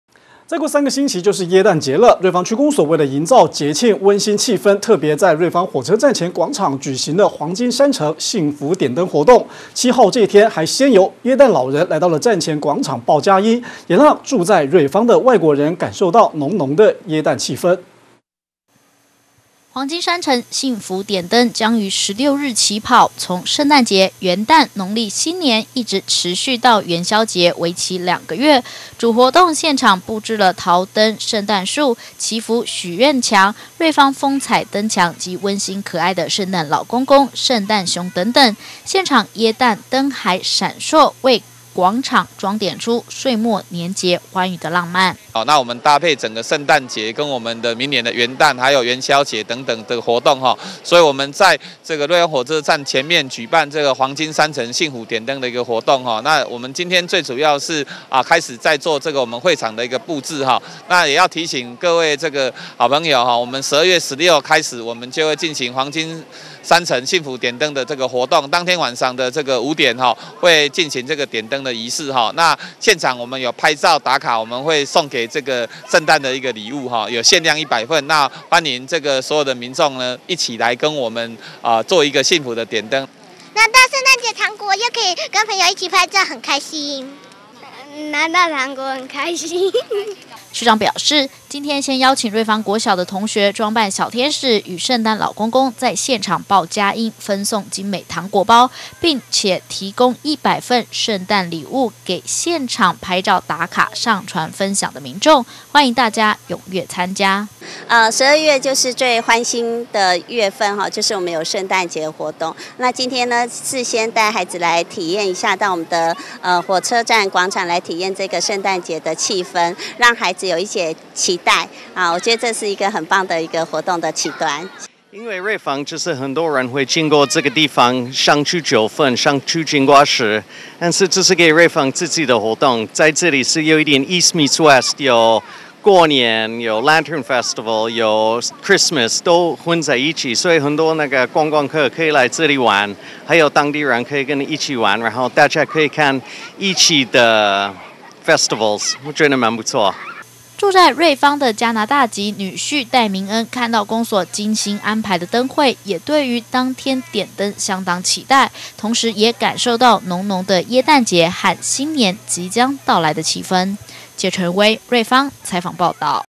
1051207觀天下新聞HD08 黃金山城幸福點燈 站前廣場報佳音
聖誕節腳步進了，瑞芳區公所為營造節慶溫馨氣氛特別於瑞芳火車站前廣場舉行「黃金山城 幸福點燈」活動，7號這天還先由聖誕老人來到站前廣場報佳音，也讓住在瑞芳的外國人感受到濃濃的耶誕氣氛。